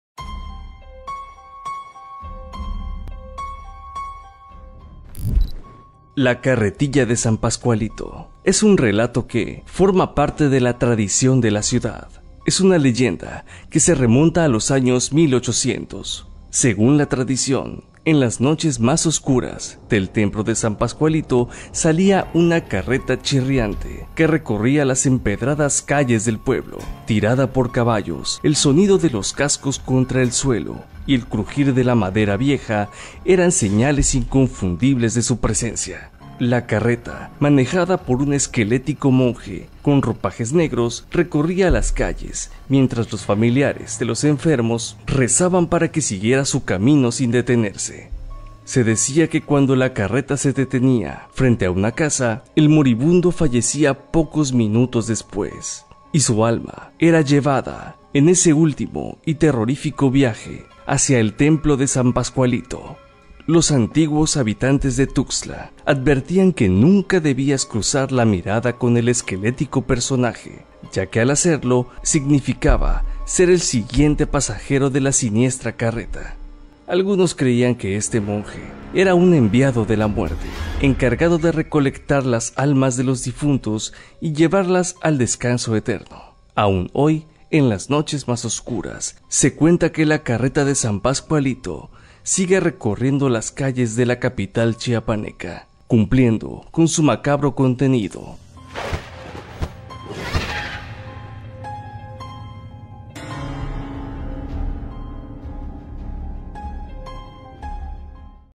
La Carretilla de San Pascualito
ElMomentoChiapas | Es un relato que, forma parte de la tradición de la ciudad de Tuxtla Gutiérrez. Es una leyenda que se remonta a los años 1800. Según la tradición, en las noches más oscuras, del templo de San Pascualito salía una carreta chirriante que recorría las empedradas calles del pueblo. Tirada por caballos, el sonido de los cascos contra el suelo y el crujir de la madera vieja eran señales inconfundibles de su presencia.

Nota: Alejandra Chávez
Voz: Miguel Blé